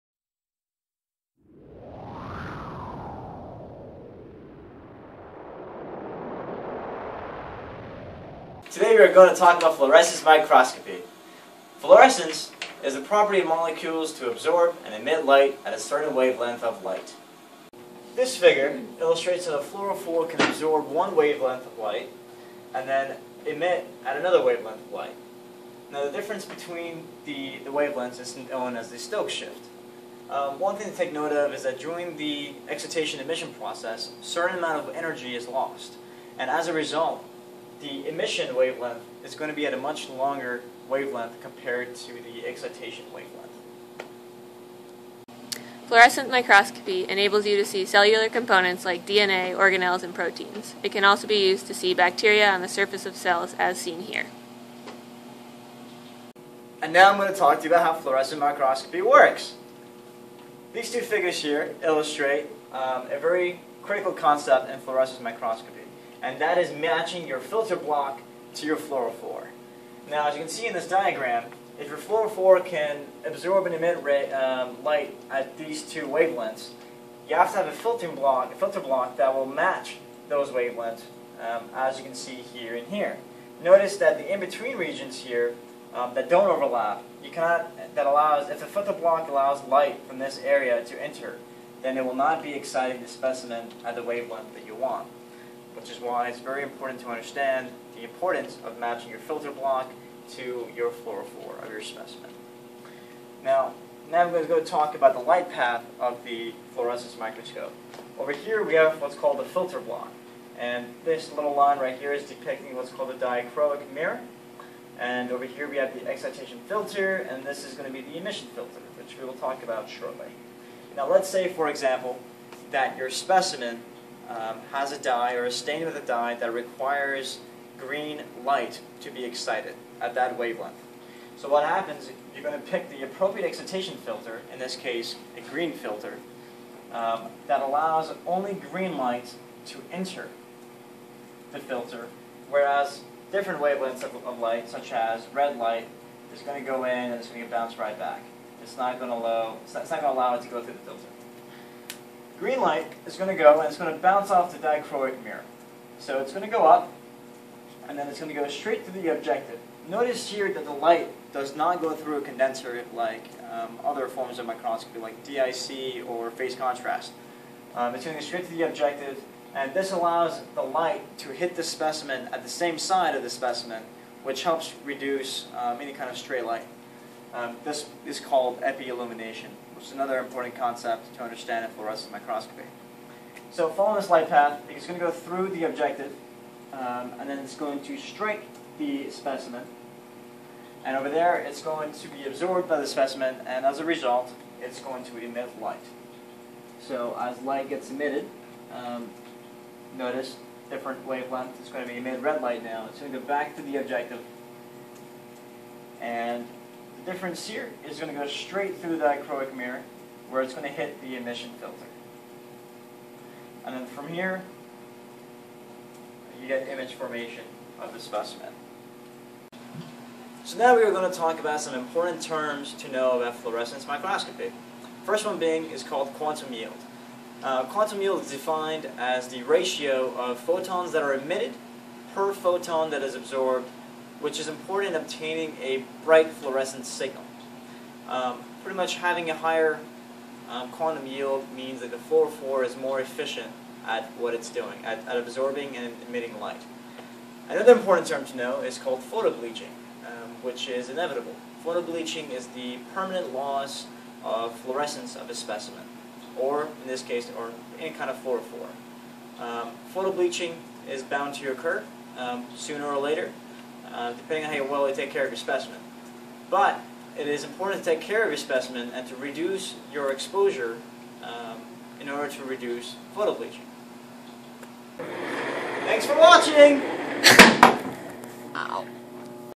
Fluorescence Microscopy in 5 mins (HD)
Talking about the basics of Fluorescence microscopy, photobleaching, quantum yield, stokes shift